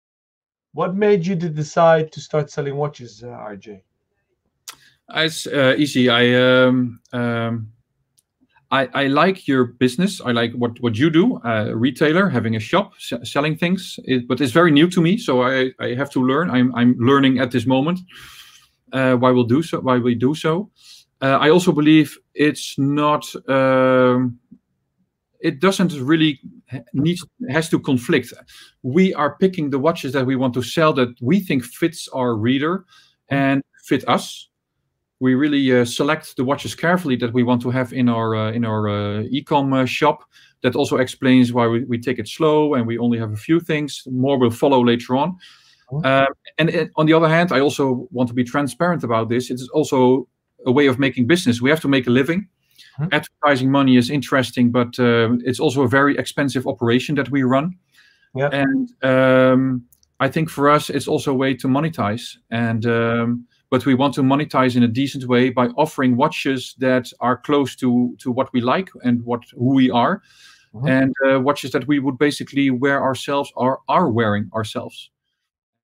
The Ace List Snippet - Selling watches on Fratello?!
Robert-Jan Broer explains why he's venturing into retail with FratelloWatches.